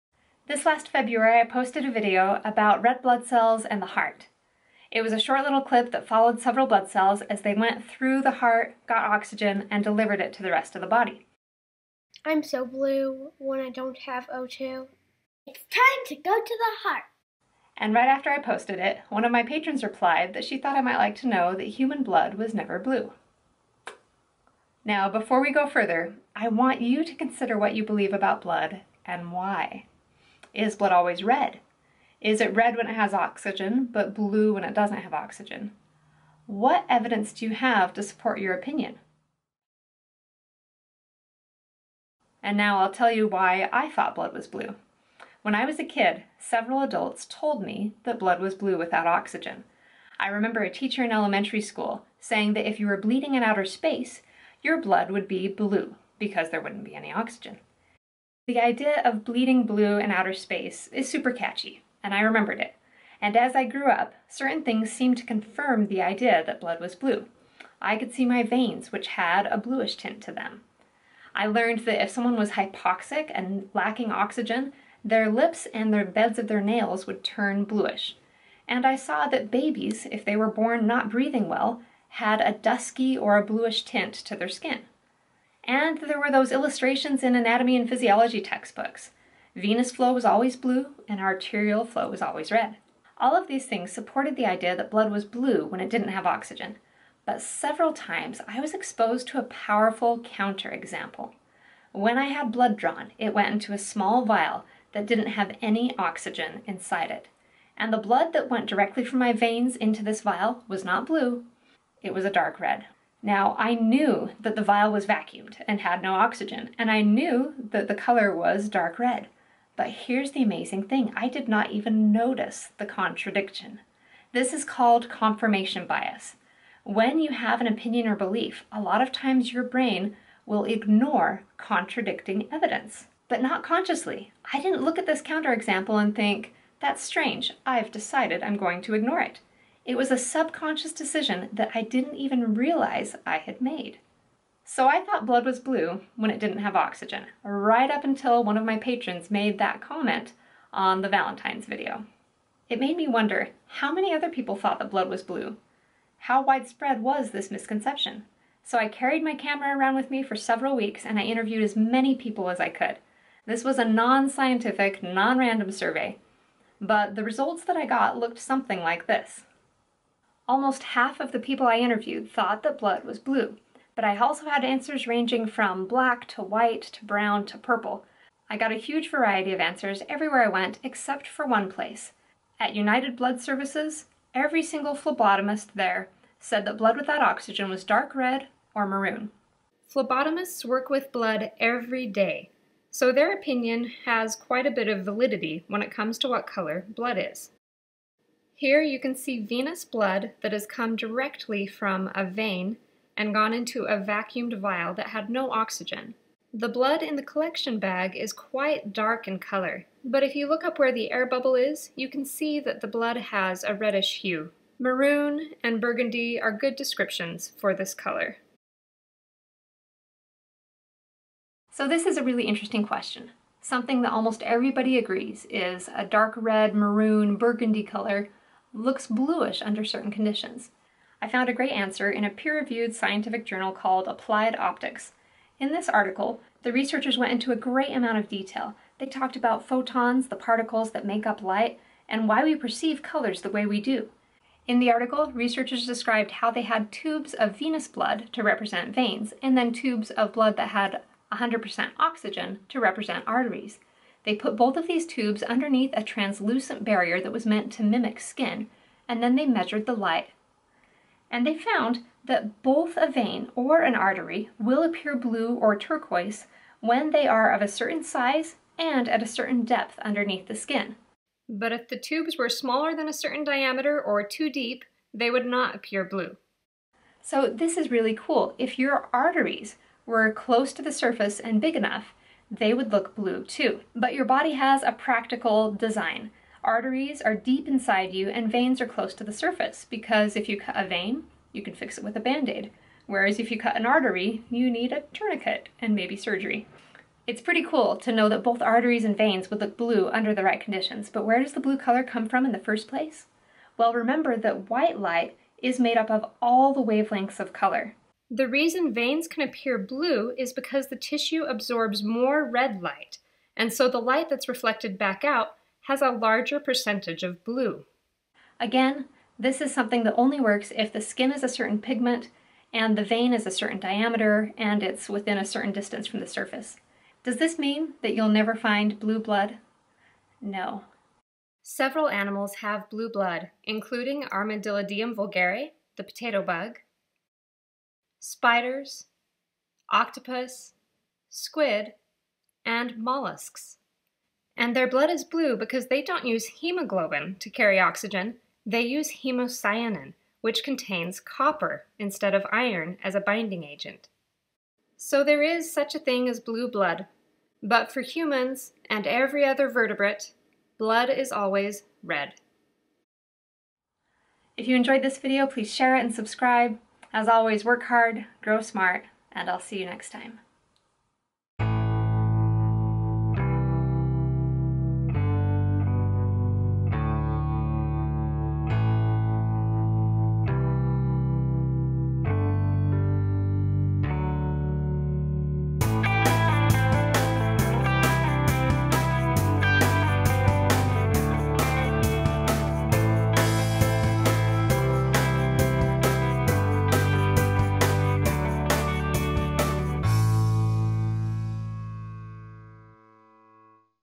What Color is Blood REALLY? (6 min version)
This is a condensed version of the original 15 minute "What Color is Blood REALLY?" video. Science Mom explores why people think blood is blue, and reveals what color blood really is -- and why veins can appear blue.